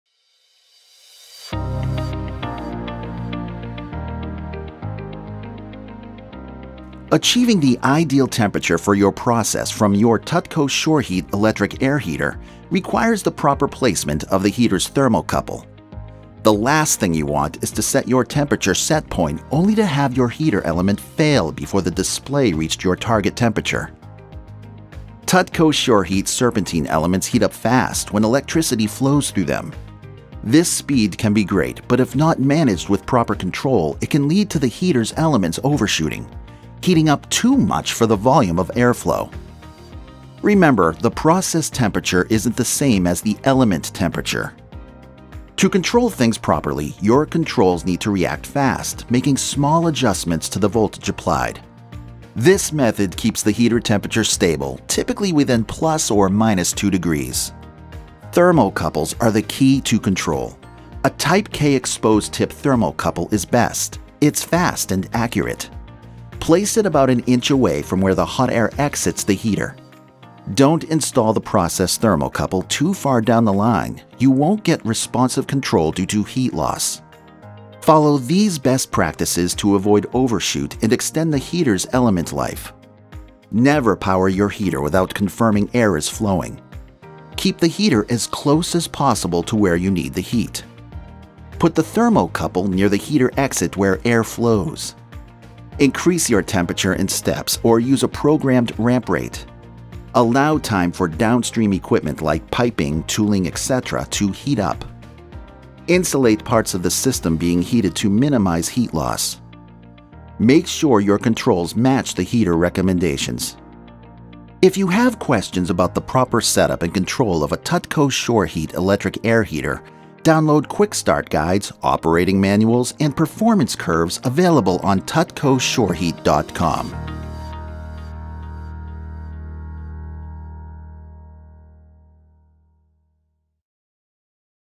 Thermocouple Placement
Achieving the ideal temperature for your process from your TUTCO SureHeat electric air heater requires the proper placement of the heater’s thermocouple. The last thing you want is to set your temperature setpoint only to have your heater element fail before the display reached your target temperature. In this video, learn a number of best practices to ensure you achieve the performance you need for your application, while avoiding overshoot and extending the heater’s element life.